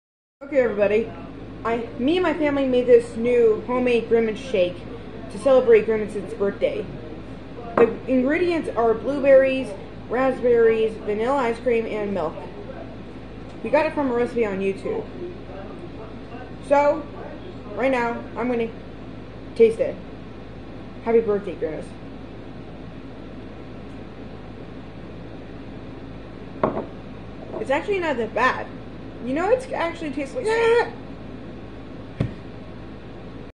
Trying the new Grimace shake for grimace's birthday.
HAPPY BIRTHDAY GRIMACE!!!